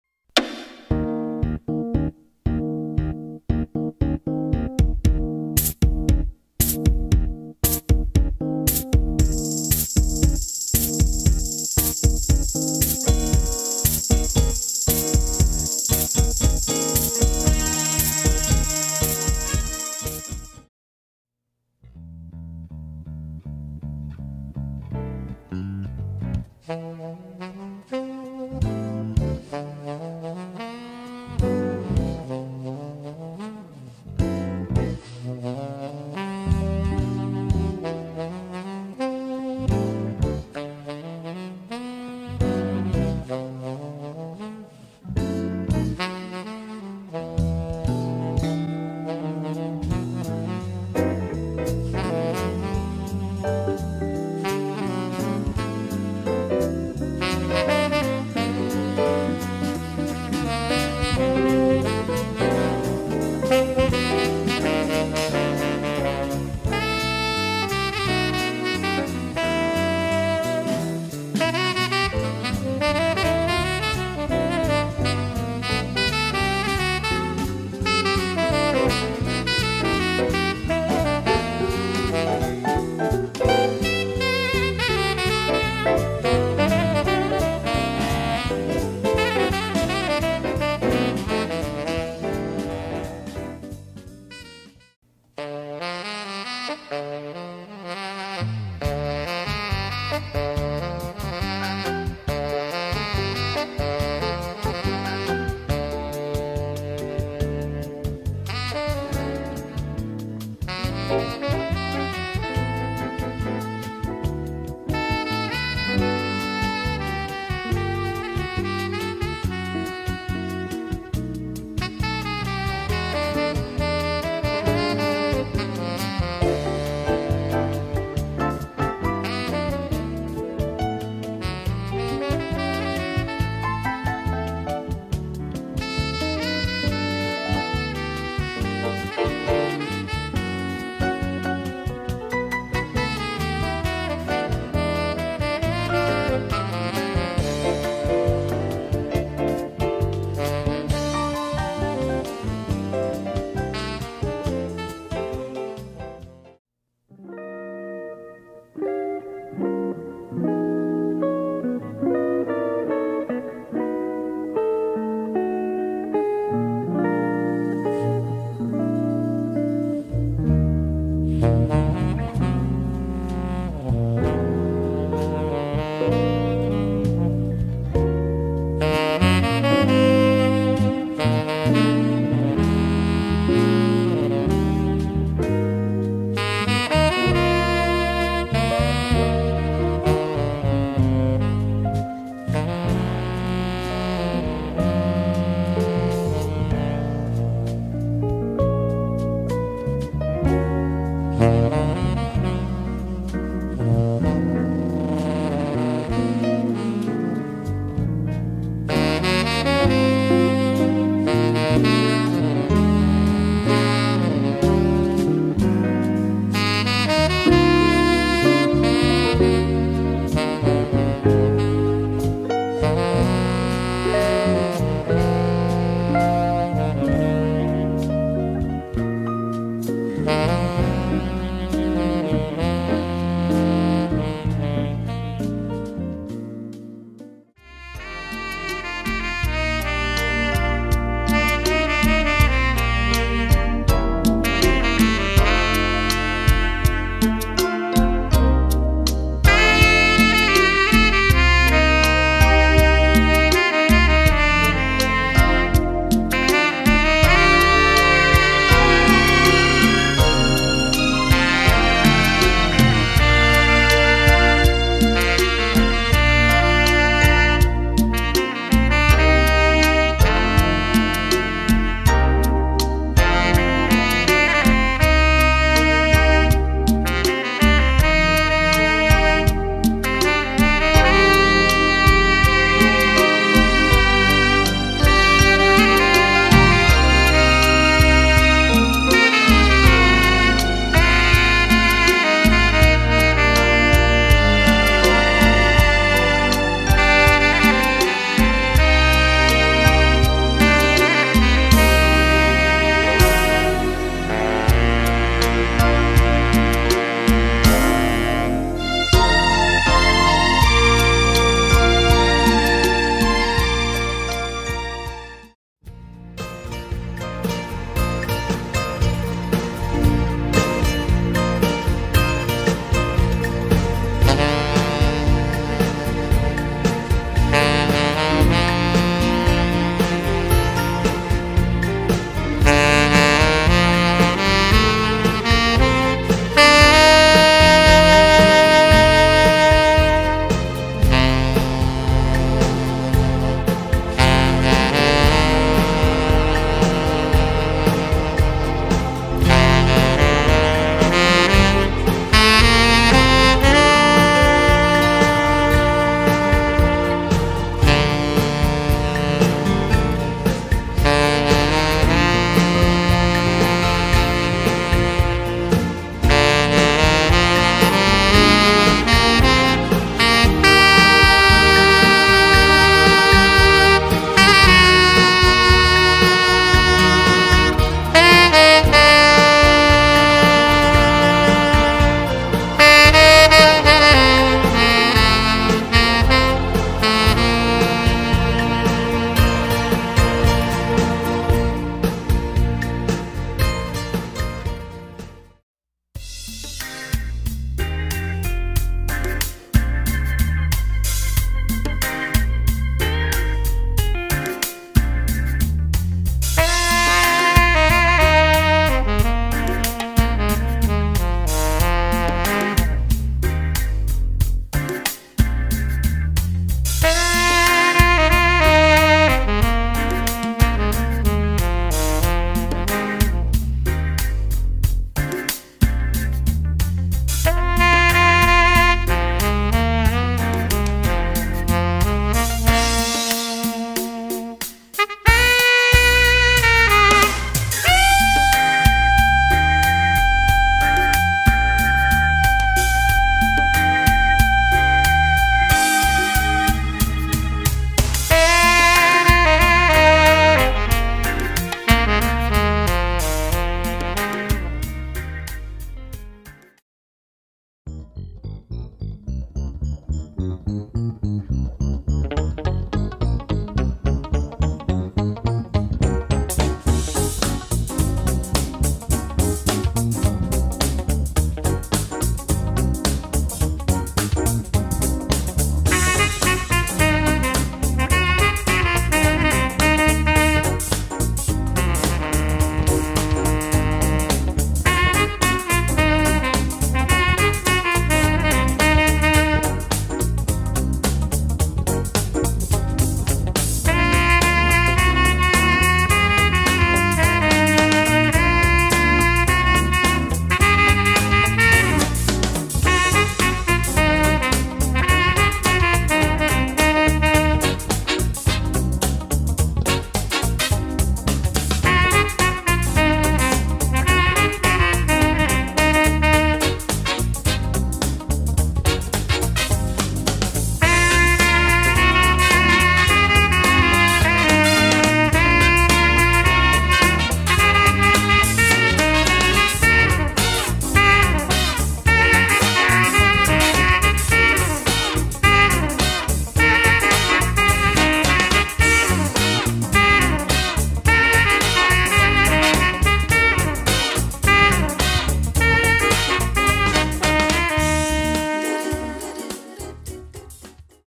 Smooth Jazz Sax - LPSaxx Demo Part II
This is Part 2 of 2 for LP Saxx. This is Lenny Peña showing his skills on the Tenor Sax.